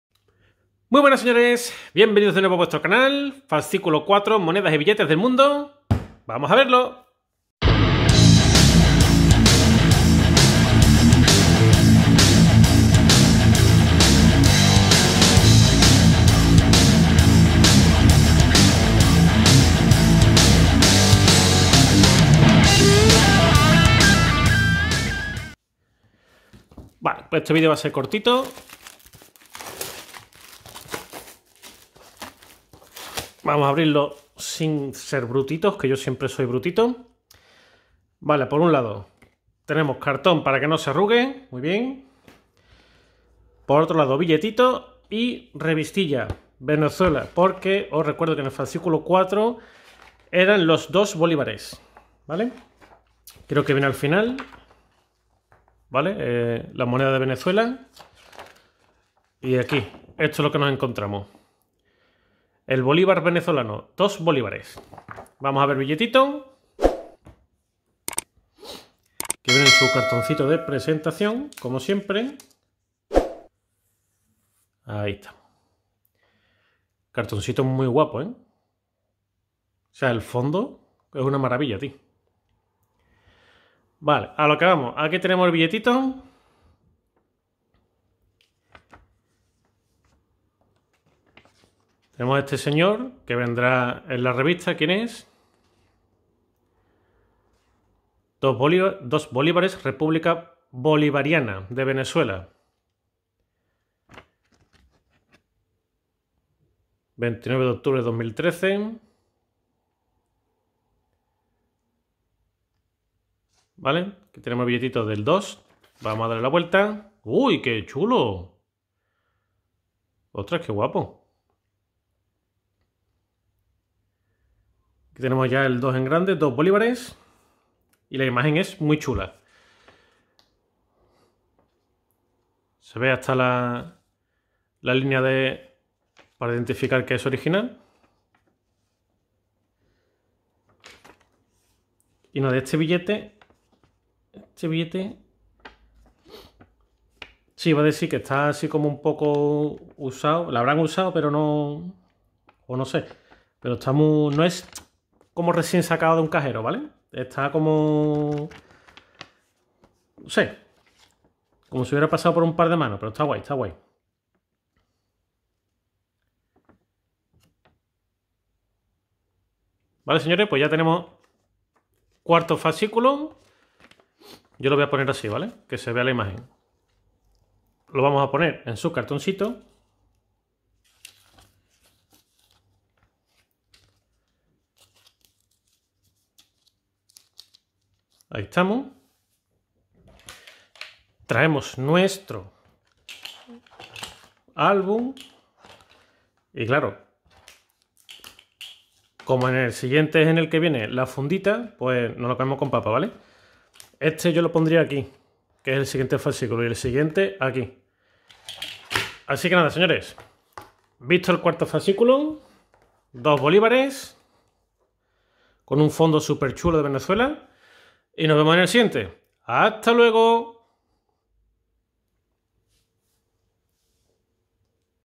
Monedas y Billetes del Mundo - 04 Venezuela: 2 Bolívares Unboxing - CajaAbierta -
Muy buenas señores.

Os dejo por aquí el fascículo 4 de Monedas y Billetes del Mundo: 2 BOLÍVARES.

- CajaAbierta -